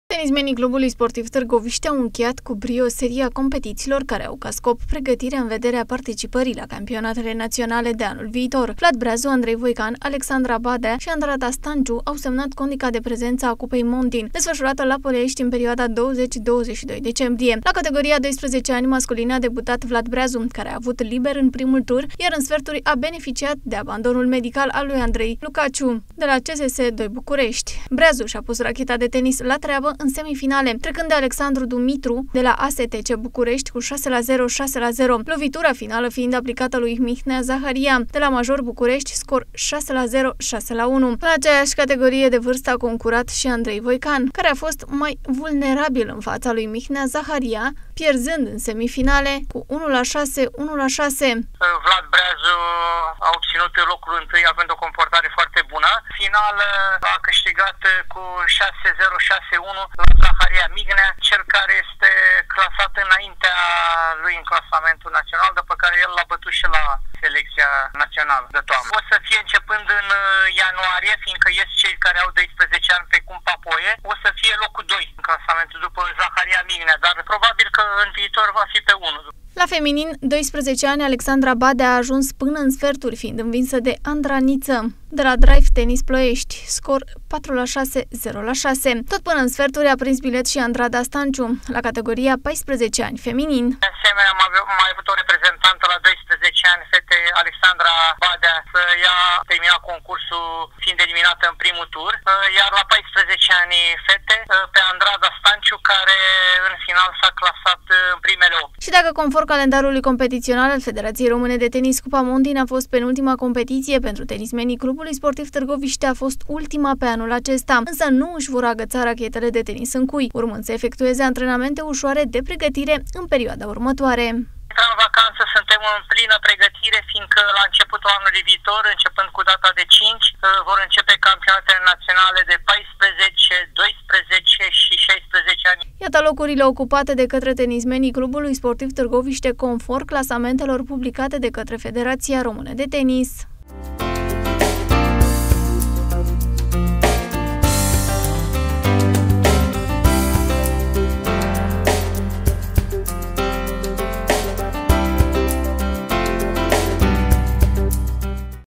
Vlad Breazu, triumfător la Cupa Montin (Columna TV)
Tenismenii CS Târgoviște au încheiat acest an competițional participând la Cupa Montin care s-a desfășurat în perioada 20-22 decembrie la Ploiești. Pe prima treaptă a podiumului am avut și de această dată un reprezentant, Vlad Breazu la categoria 12 ani.